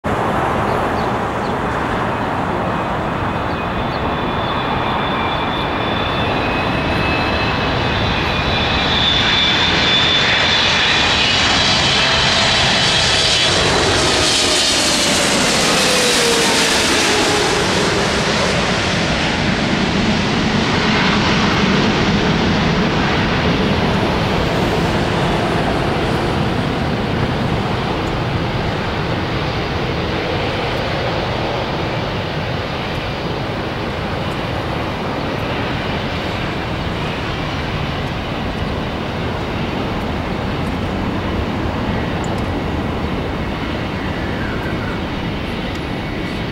Turkish Airlines TC-JJH 777-300ER Landing Los Angeles Airport (LAX)
Flight: THY9 IST-LAX